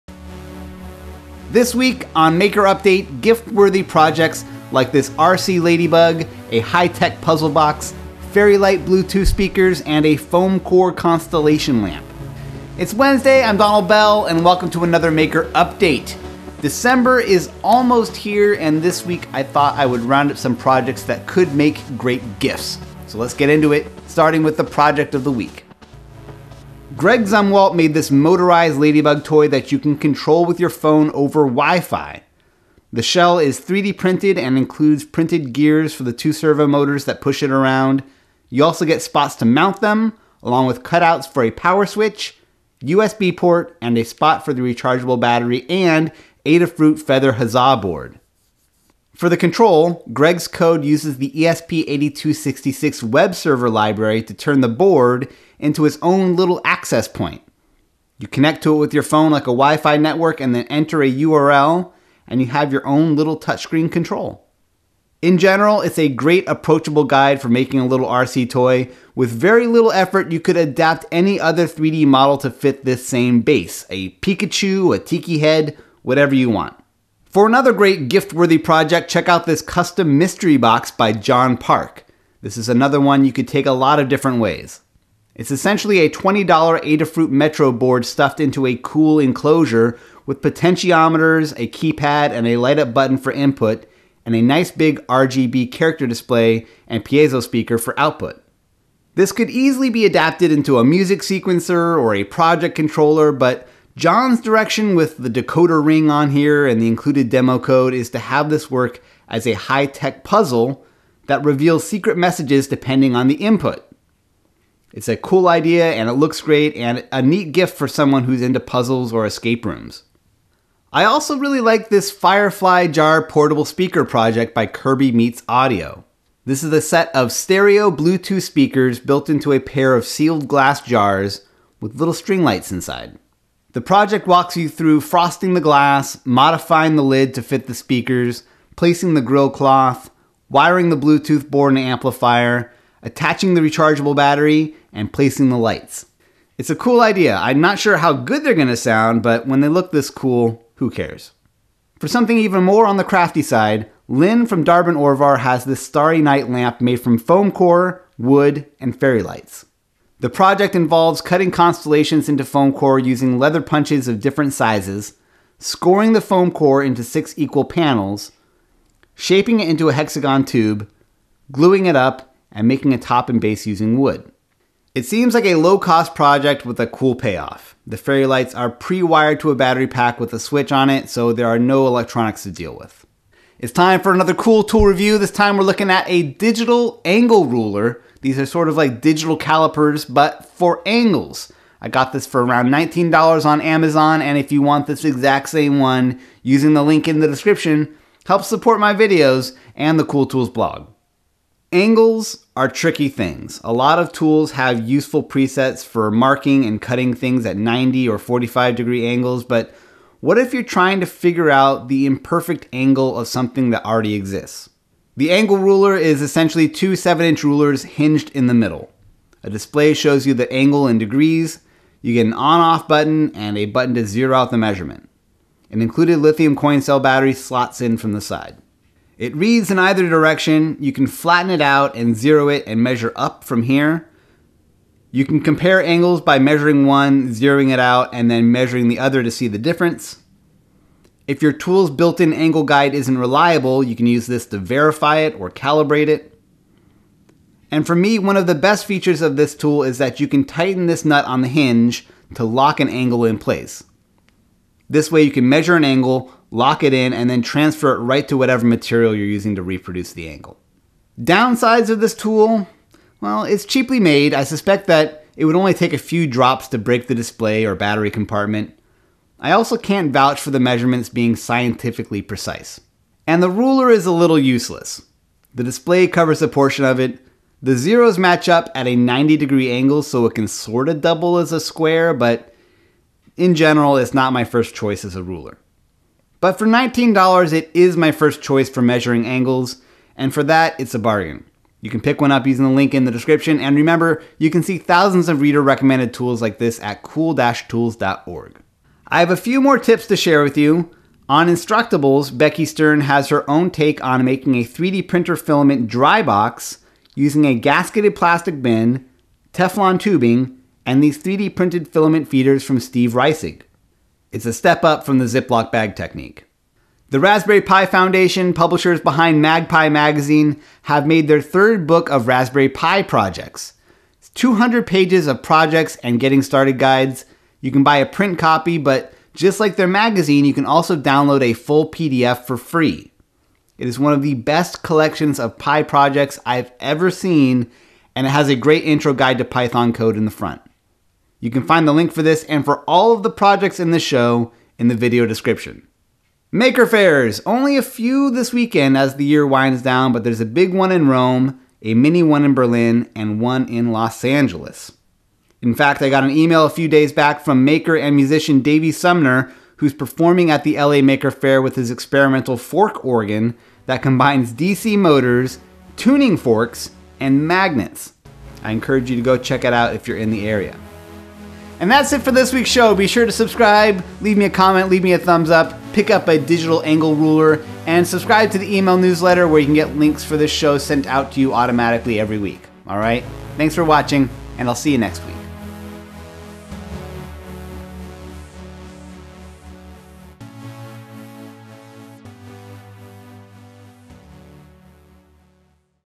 All the Angles [Maker Update #62]
This week, gift-worthy projects including an RC Ladybug, high-tech puzzle box, firefly jar Bluetooth speakers, and a foam core constellation lamp. This week’s Cool Tool is a Digital Angle Ruler

++Show Notes++

-=Advanced Project=-

-=More Projects=-

Magical Starry Night Light W/ Wood, Foamboard & Fairy Lights by darbinorvar

Digital Angle Ruler

-=Tools/Tips=-
3D Printer Filament Dry Box by bekathwia

-=Maker Faires=-
Rome Italy
Berlin Germany